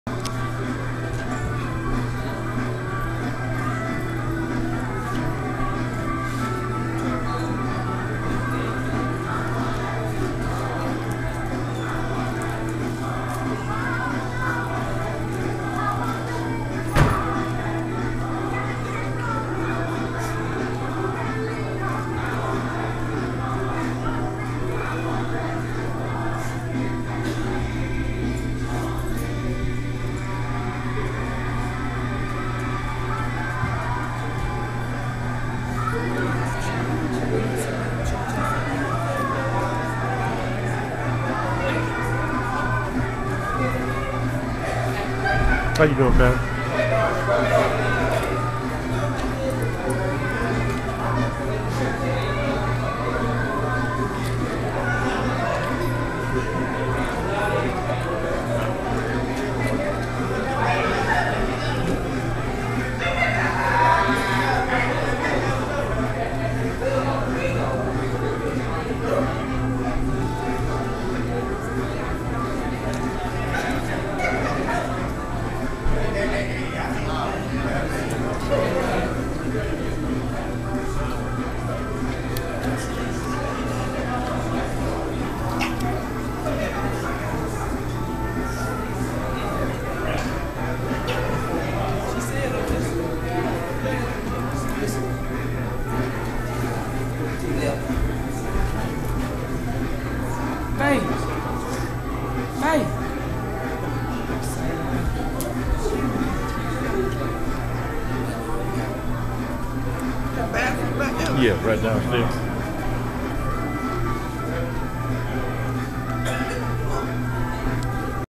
Pastor Joe Powell St James Freewill Baptist Church Homegoing Celebration #3
@ New Providence Baptist Church Fitzgerald,Ga 3-20-12